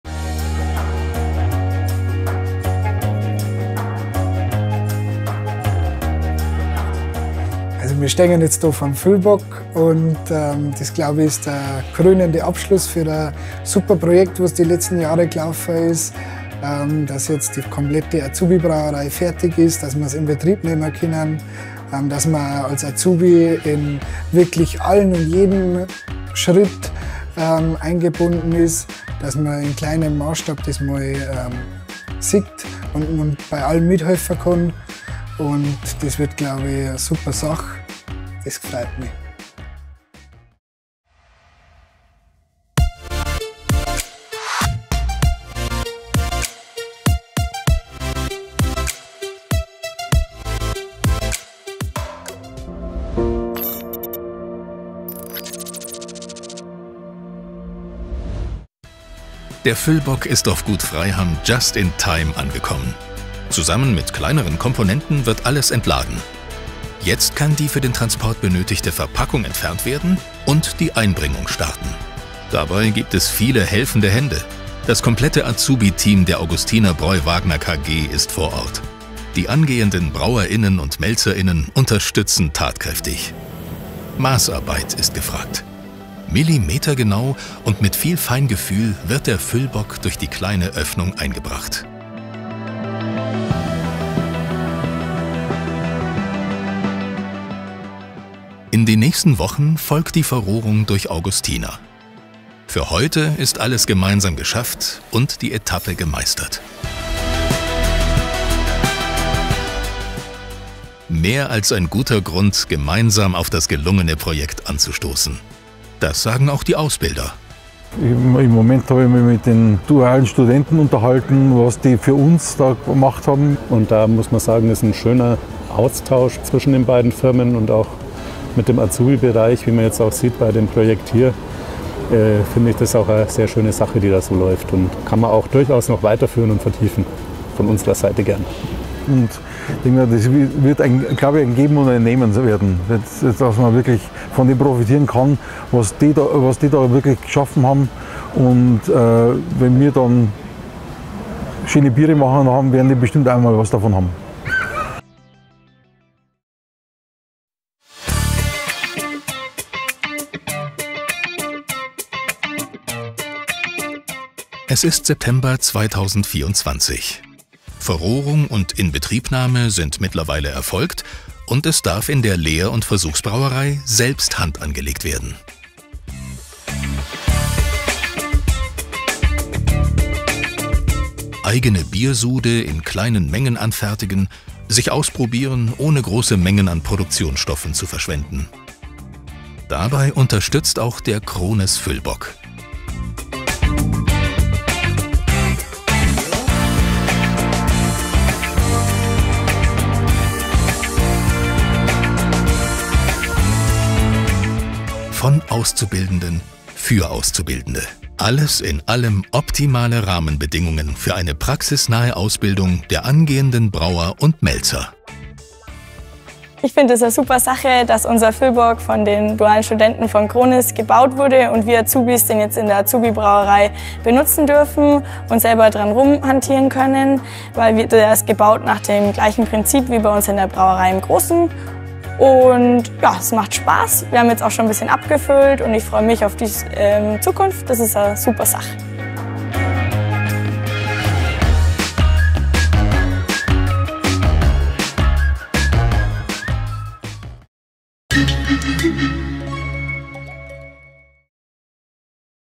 Von Azubis für Azubis: Das Füllbock Projekt zwischen Augustiner-Bräu und Krones. Teil 2
In dem zweiten Teil des Füllbock Projekts zwischen Augustiner-Bräu und Krones wird die erfolgreiche Inbetriebnahme des Füllbocks in der neuen Lehr- und Versuchsbrauerei auf Gut Freiham vorgestellt. Auch bei der Installation sind die Azubis wieder aktiv beteiligt.
Schaut den Auszubildenden dabei zu, wie sie nun eigene Biersude in kleinen Mengen produzieren und abfüllen können und dabei wertvolle Erfahrungen sammeln.

Und vieles mehr aus der Welt der Krones AG bieten wir in dem Krones magazine